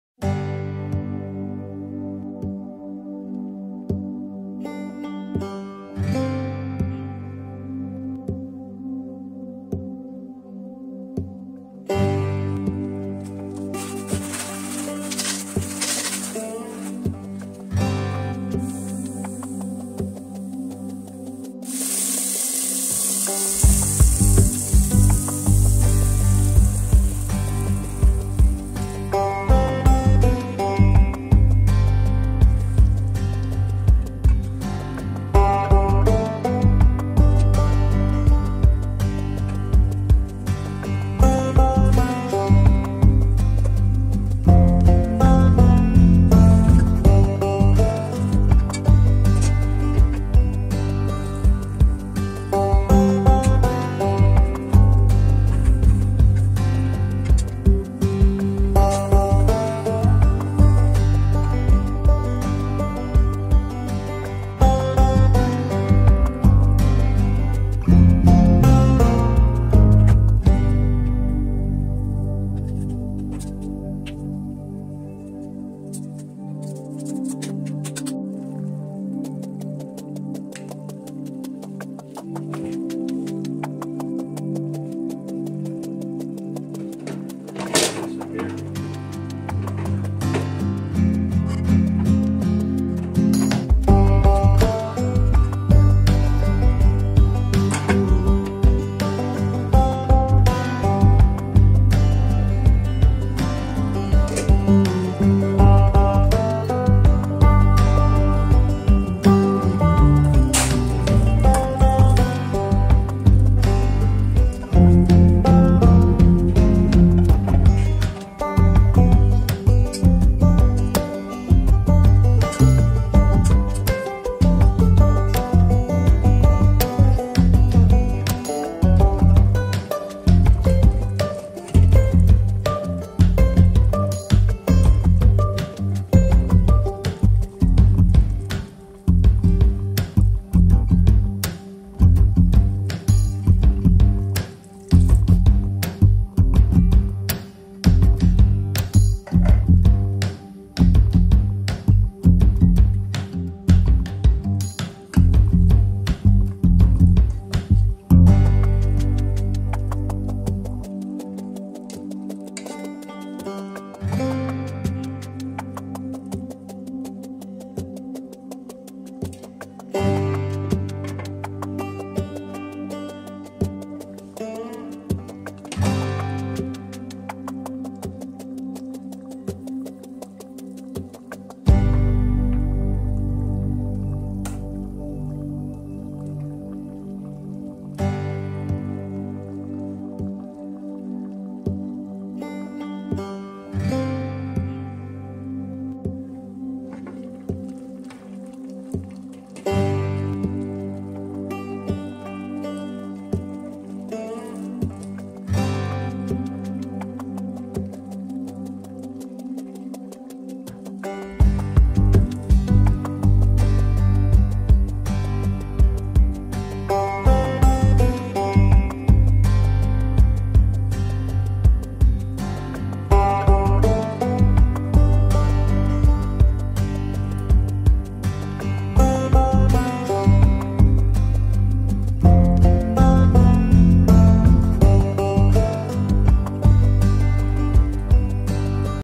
How to cook jumbo shrimp with coconut milk Bisaya style
pinoy food, paano mag luto ng shrimp, shrimp with gata, shrimp with coconut